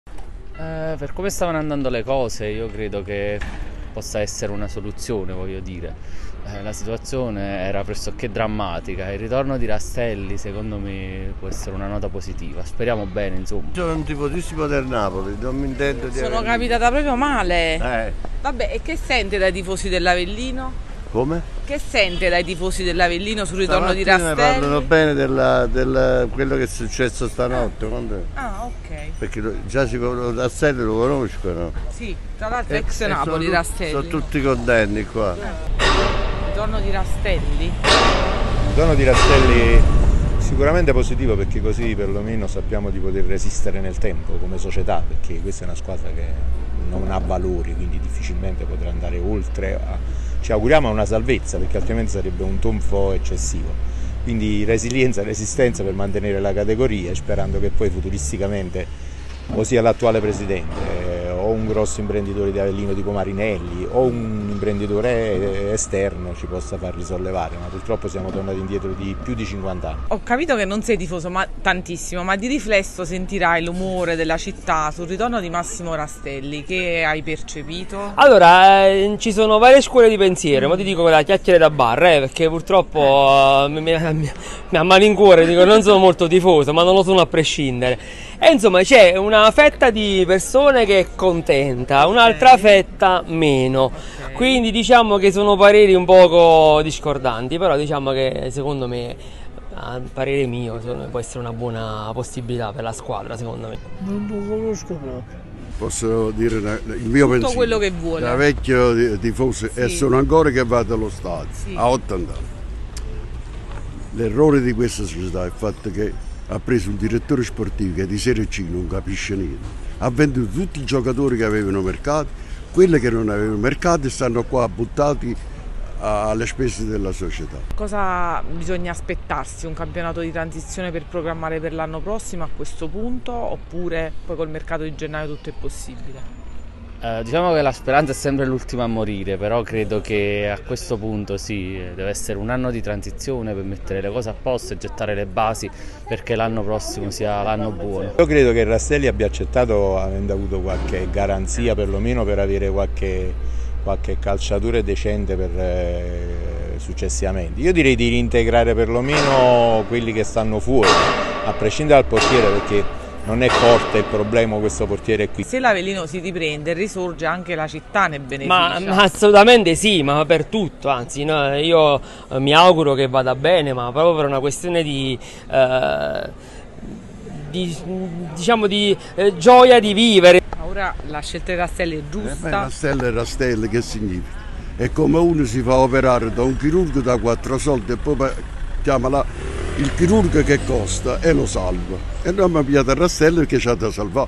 La voce dei tifosi: "Rastelli è il chirurgo che rianimerà i tifosi"
Così dopo la conferma del ritorno in Irpinia del tecnico di Pompei, fiducia massima per risollevare le sorti del lupo